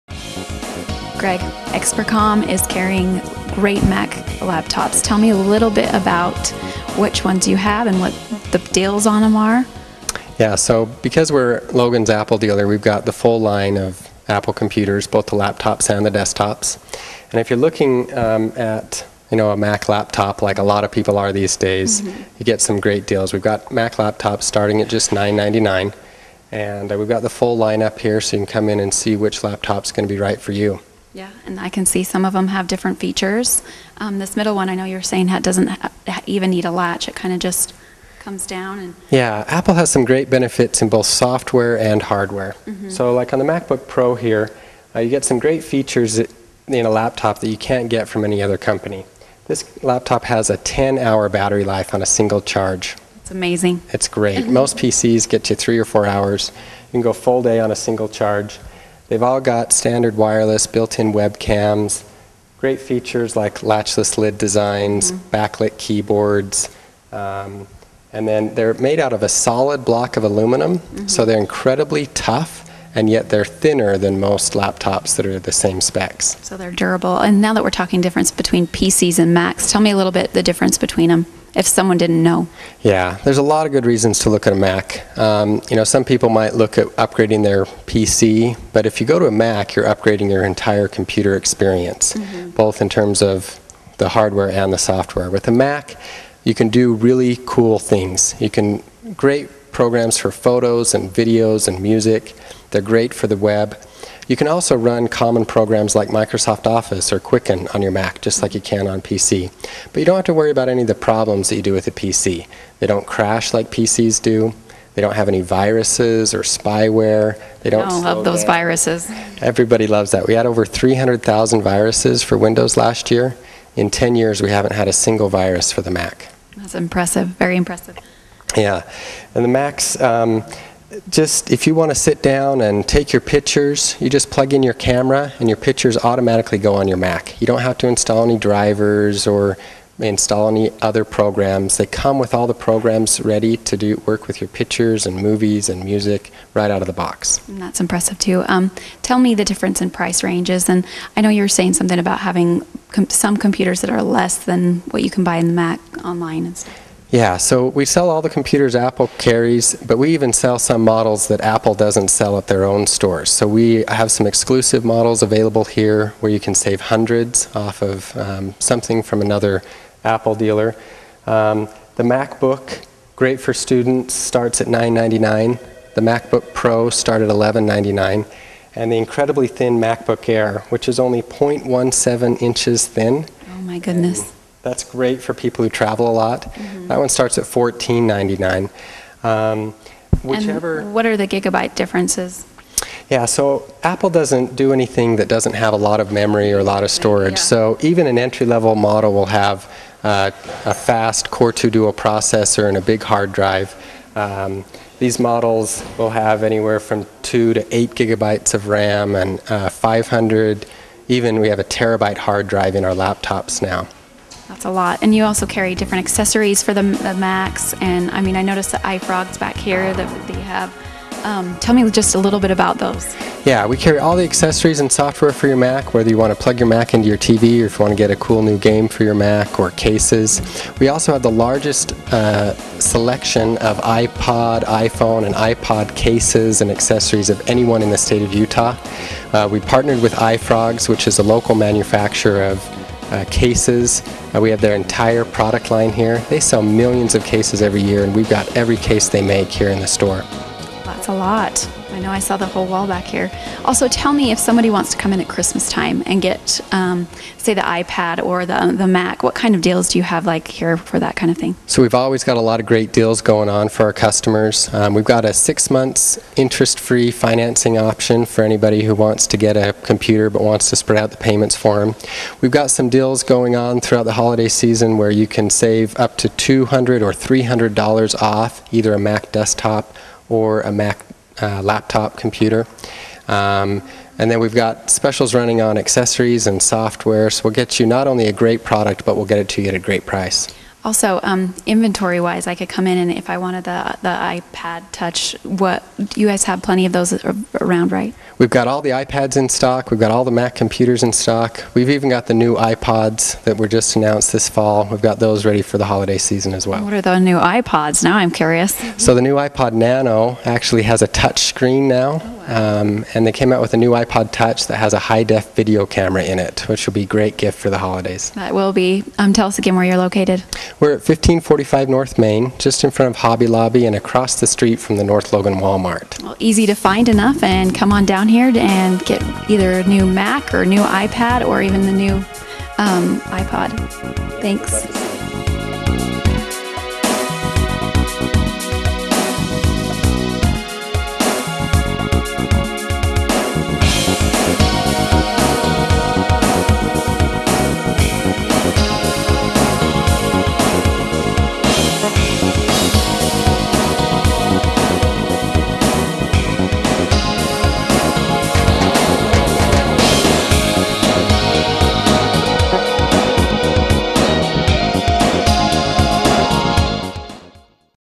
Expercom laptops
Expercom is the Apple store in Logan Utah, They always have great deals on laptops and Mac computers